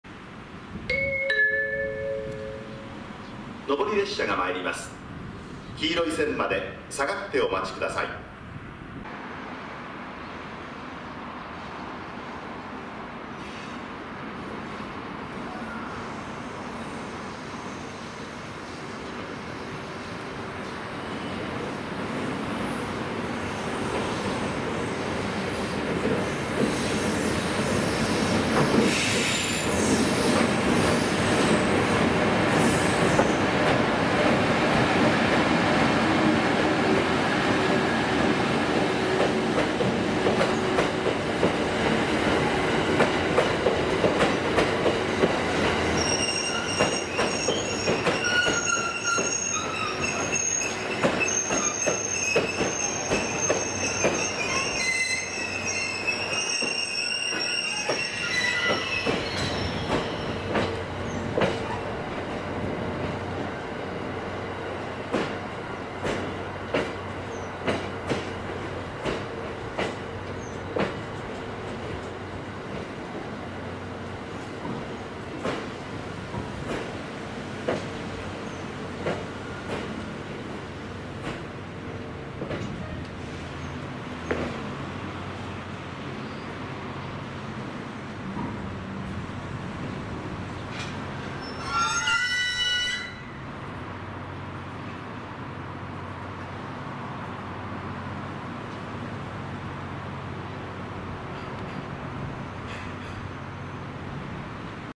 カシオペア紀行[青森]返却回送2020📹
🟥EF81 80号機💫牽引
回9110レ🚉浅虫温泉駅にて
2020年11月22日撮影